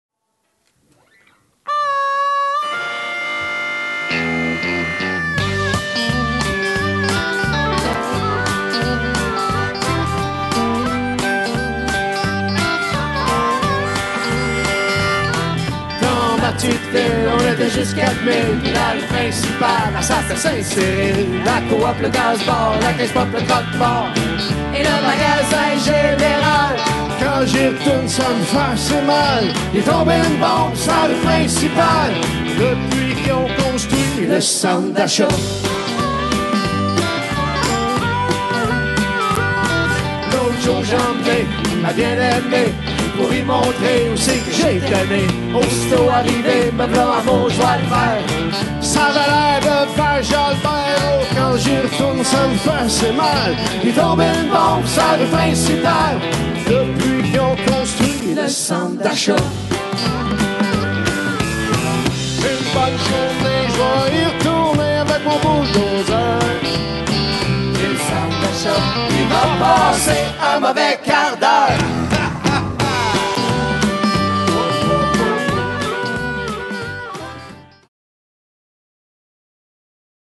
La Fête nationale à Montréal en répétition
Le grand spectacle de la fête nationale à Montréal en répétition.
Un extrait avec Richard Séguin